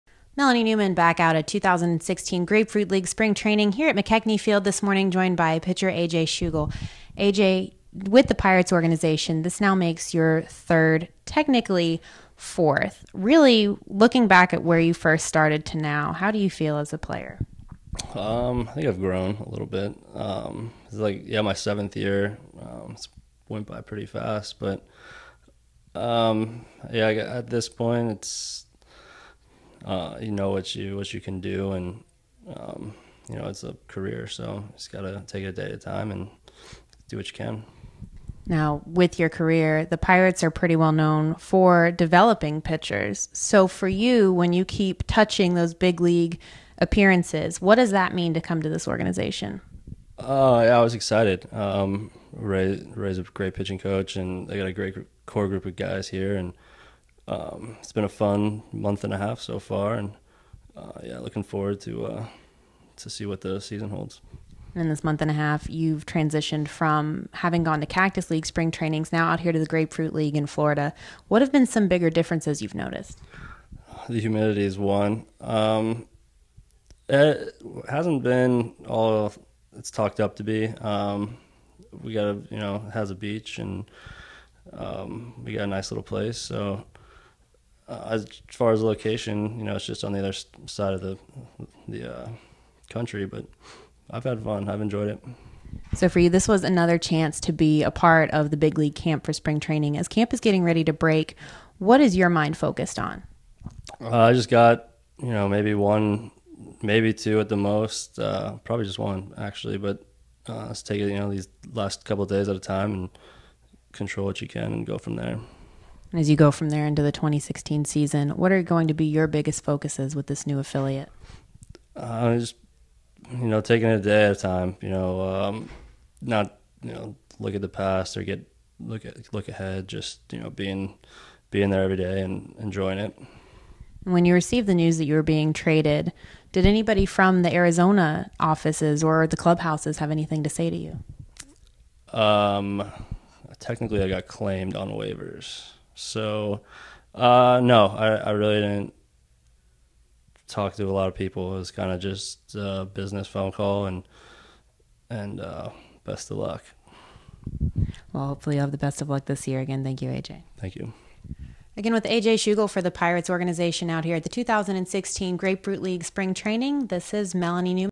A.J. Schugel at Spring Training
A.J. Schugel on his new season with the Pirates, Grapefruit v Cactus and more